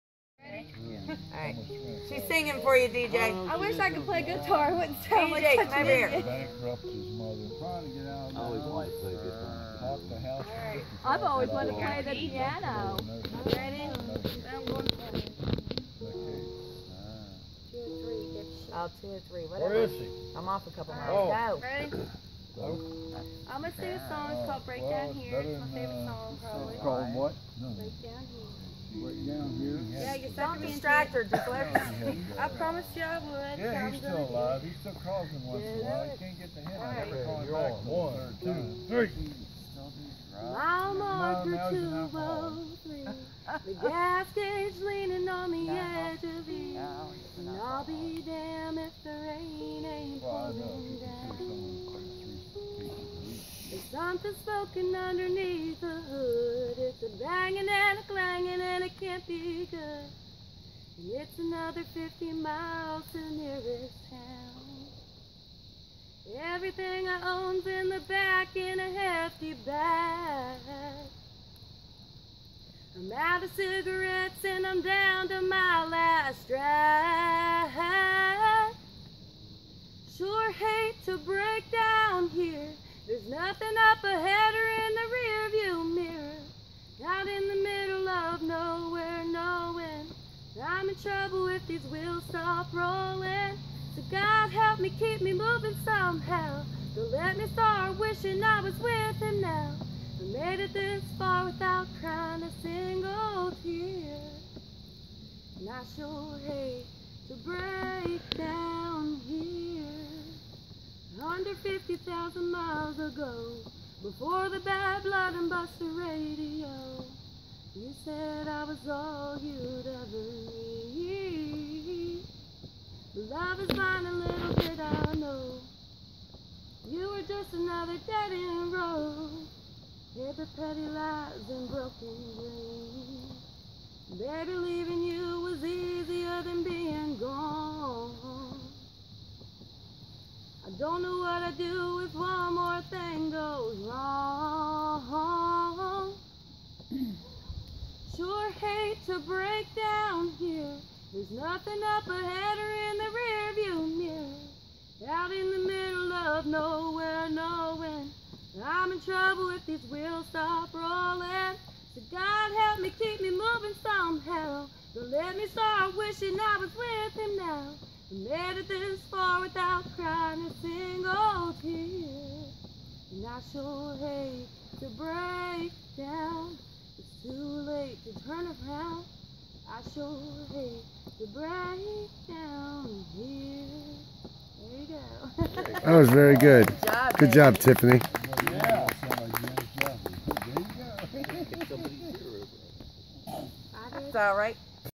Tiffany Singing At Family Reunion
Tiffany,  Daughter of Mariah, singing at campfire at the family reunion 5/24/15